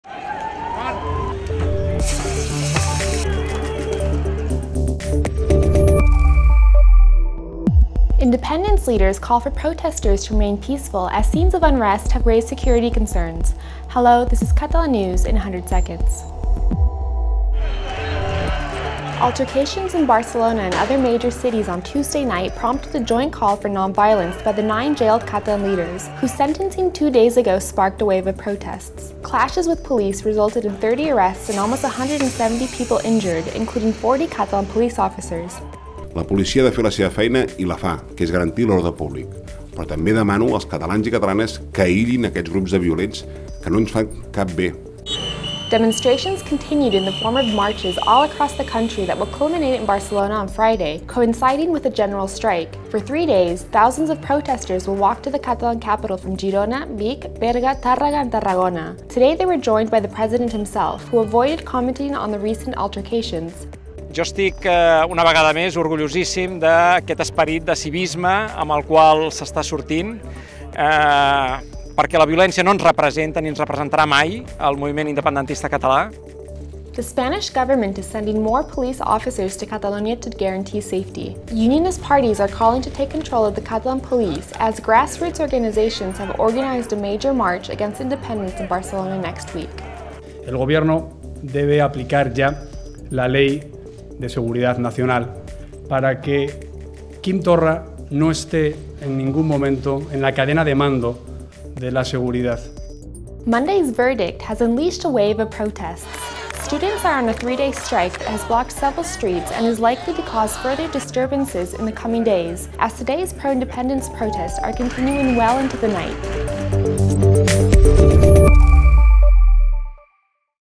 16-Oct-19: 'Independence leaders call for protesters to remain peaceful'
Altercations in Barcelona and other major cities on Tuesday night resulted in 30 arrests and almost 170 people injured, including 40 Catalan police officers